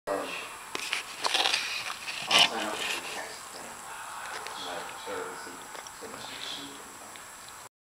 Supreme pizza
We were eating pizza a couple of hours before the Super Bowl.